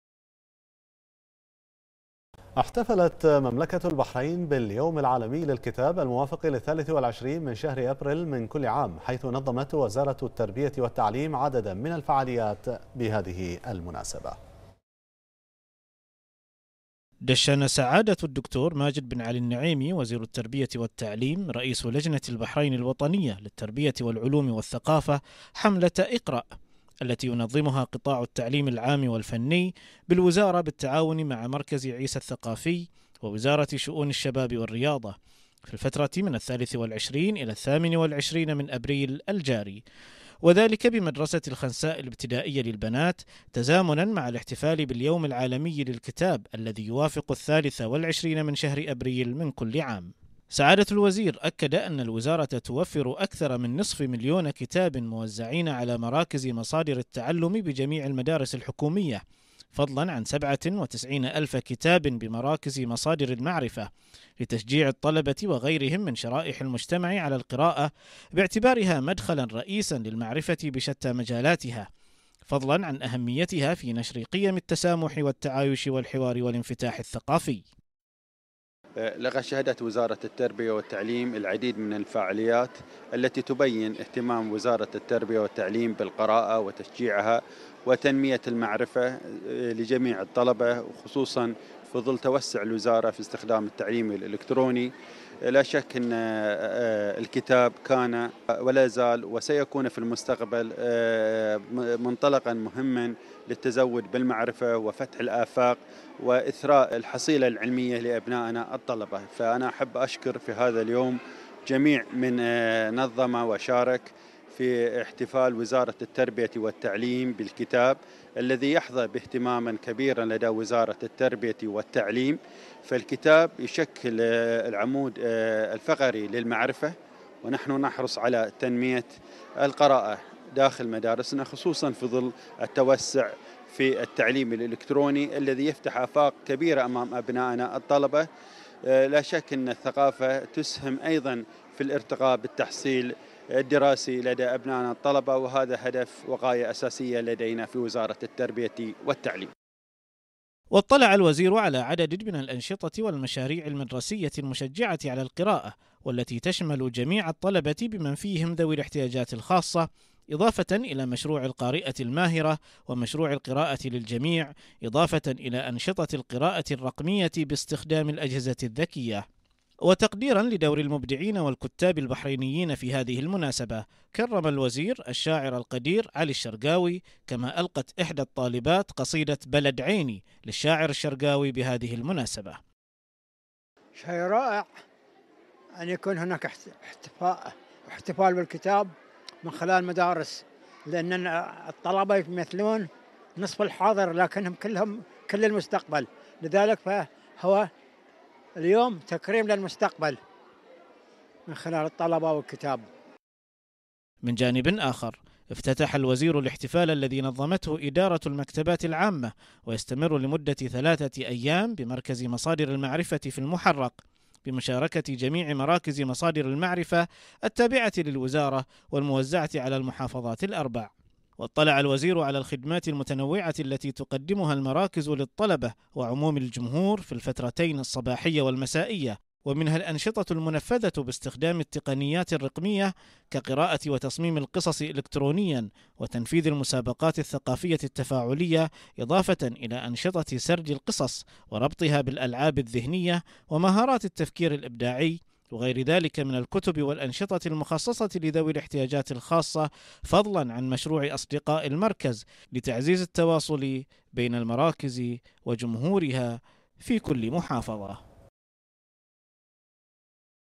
البحرين: وزارة التربية والتعليم تحتفل باليوم العالمي للكتاب
المنامة في 23 ابريل /بنا/ احتفلت مملكة البحرين باليوم العالمي للكتاب الموافق للـ23 من أبريل من كل عام، حيث نظمت وزارة التربية والتعليم عدداً من الفعاليات بهذه المناسبة.